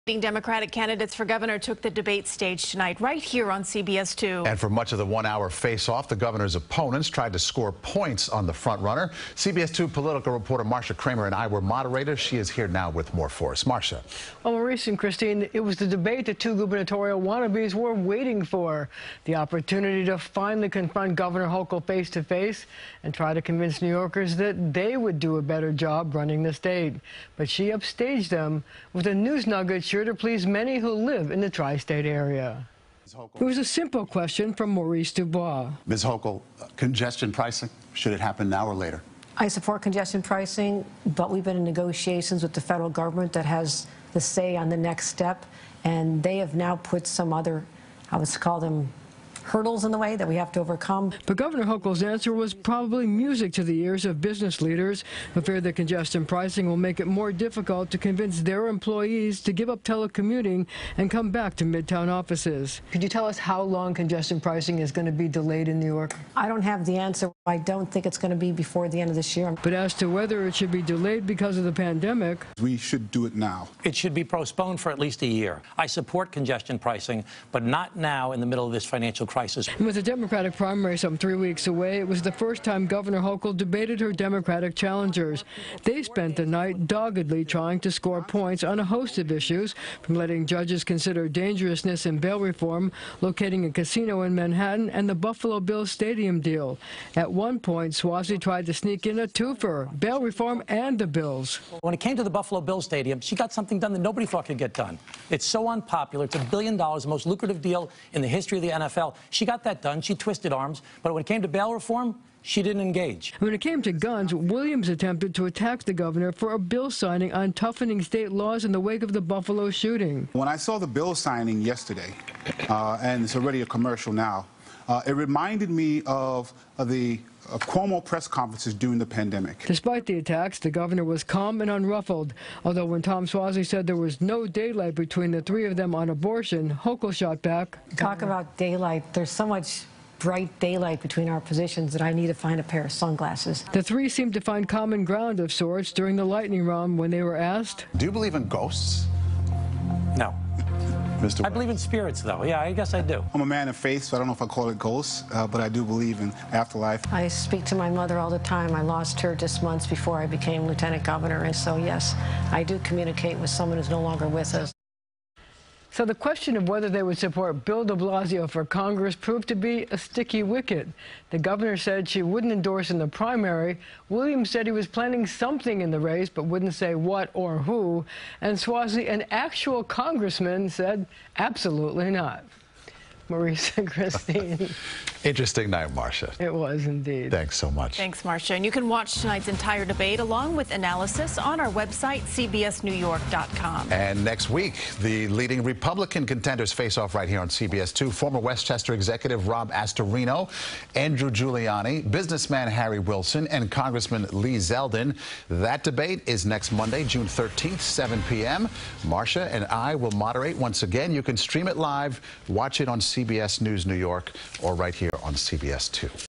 Possible delay to congestion pricing comes out of spirited Democratic gubernatorial debate
Congressman Tom Suozzi and Public Advocate Jumaane Williams tried to unnerve Gov. Kathy Hochul throughout the hour-long debate. CBS2's Marcia Kramer has a recap.